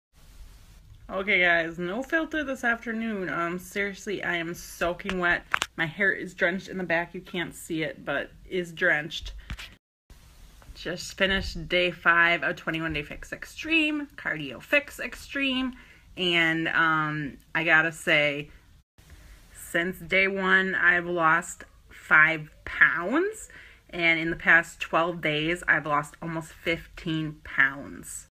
Progress after 5 days of 21DFX
OMG! Just finished Day 5/21 of 21 Day Fix Extreme: Cardio Fix X! I used 8 and 12.5 pounders ... progress! I've lost 5 pounds in the last 5 day of 21DFX and 14.4 pounds after 12 days of my new WayOfEating !!! I know cutting carbs doesn't work for everyone, but it's working for me!!! About 15% carbs, 20% protein, and 65% healthy fats seems to be my sweet spot! 😉❤️💪